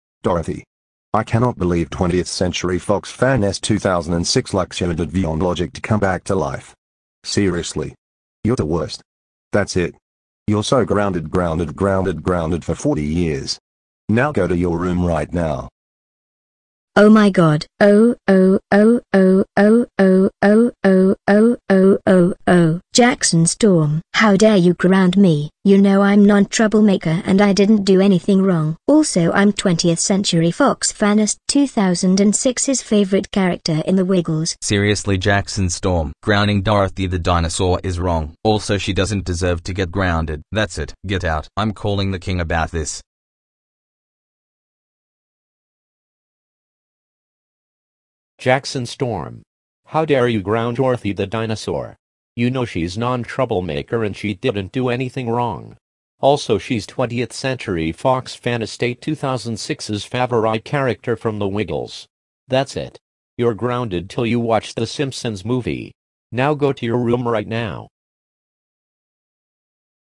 Jackson Storm Grounds Dorothy The Dinosaur And Gets Grounded By The King
Requested By @kanebrenchley
Also sorry for not having the Dallas voice for the King. So I used Tom instead